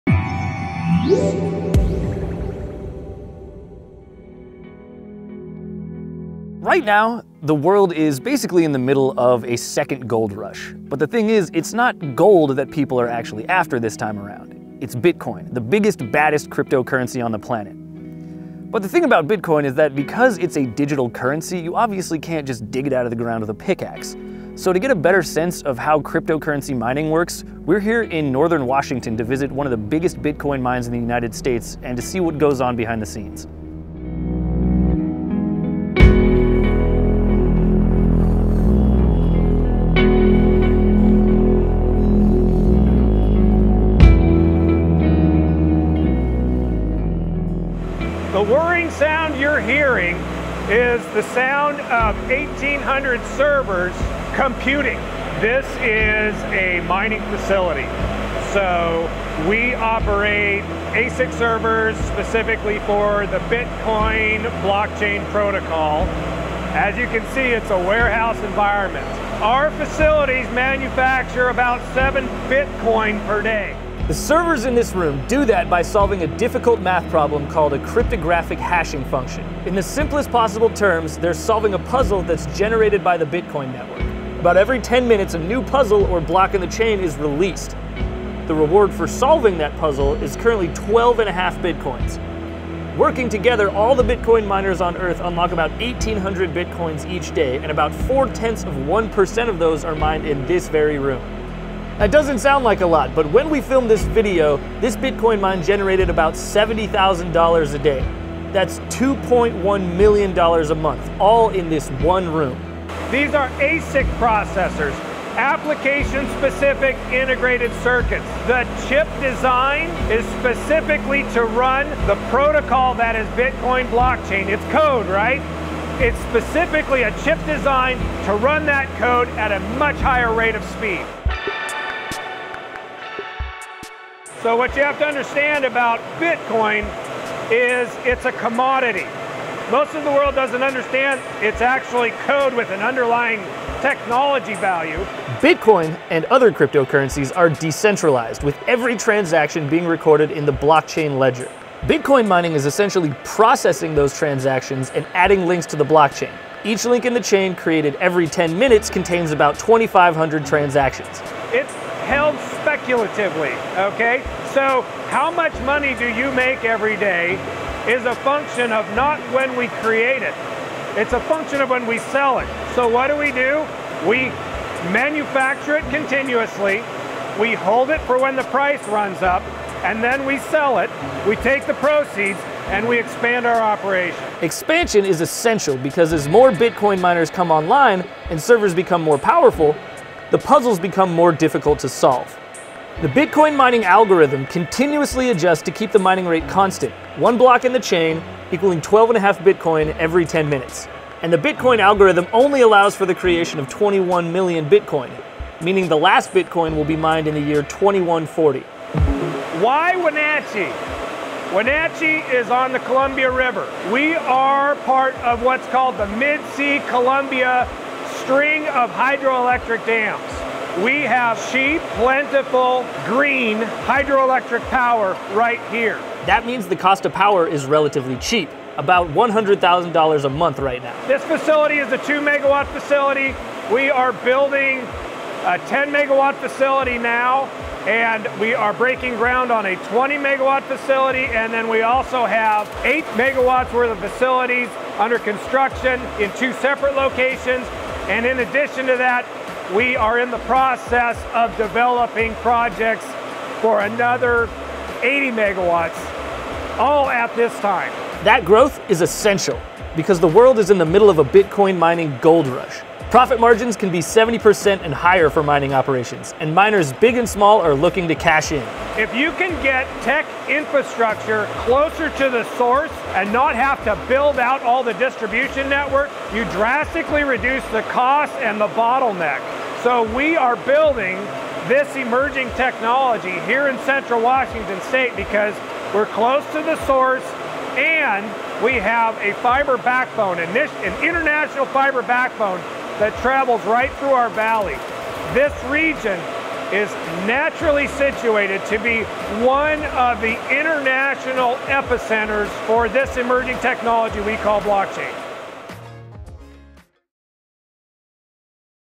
Inside a Bitcoin mine that earns $70K a day
The virtual goldrush to mine Bitcoin and other cryptocurrencies leads us to Central Washington state where a Bitcoin mine generates roughly $70,000 a day mining bitcoin. A warehouse full of ASIC Bitcoin miners run 24/7 to manufacture Bitcoin.

The location of this Bitcoin Mine is one of the world's epicenters for mining. Cheap hydroelectric power increases margins and a major fiber backbone runs mere miles from the mine.